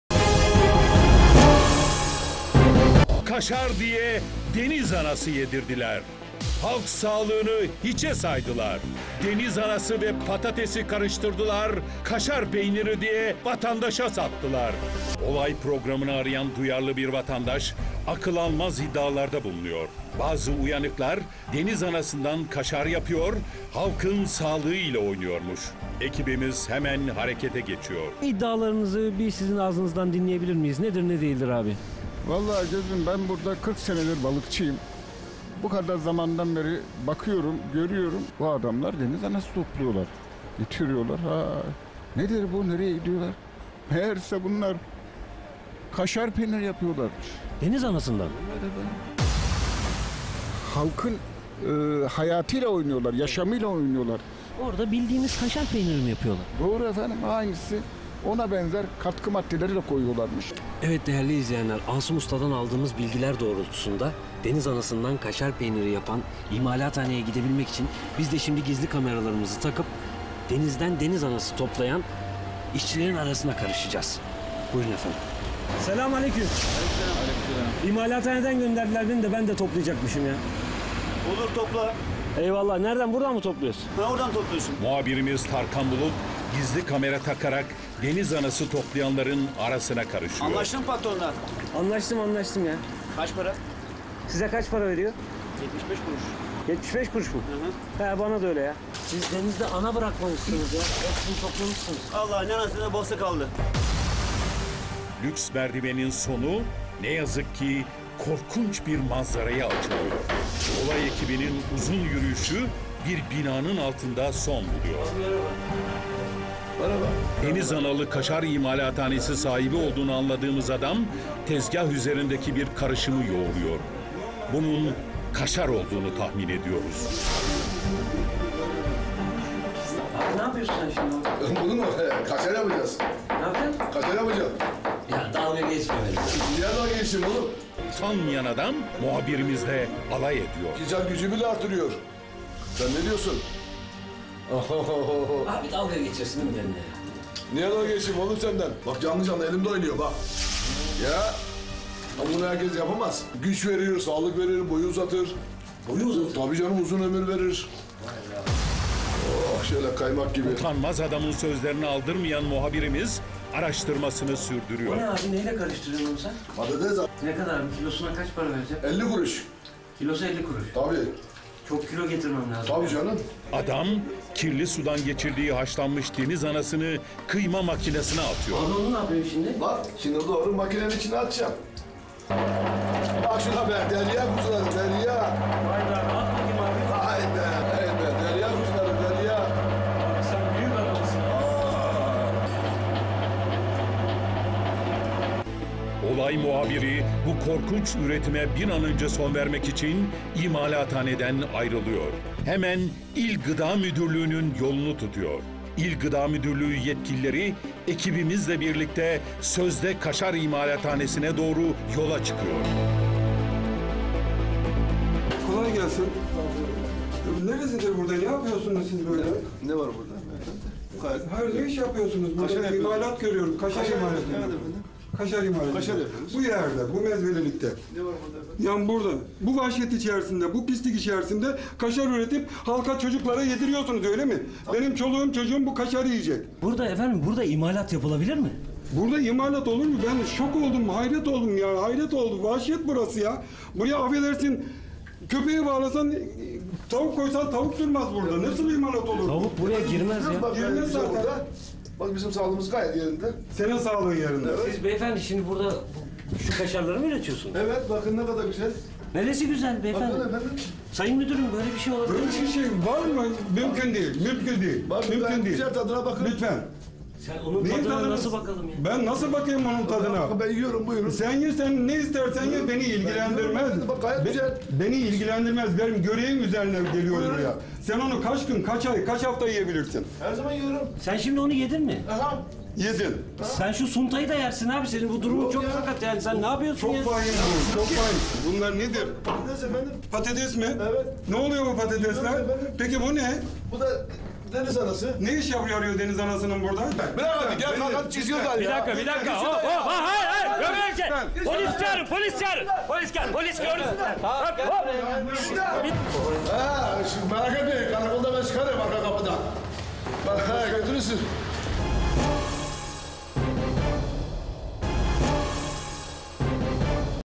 Deniz Anasından Kaşar Yaptılar!
İl Gıda Müdürü'nü hayrete düşüren olay: ''Analarımızın tostumuza koyduğu kaşarlar deniz anası çıktı!''

Bu video IMM tarafından mizah amacı ile üretilmiştir.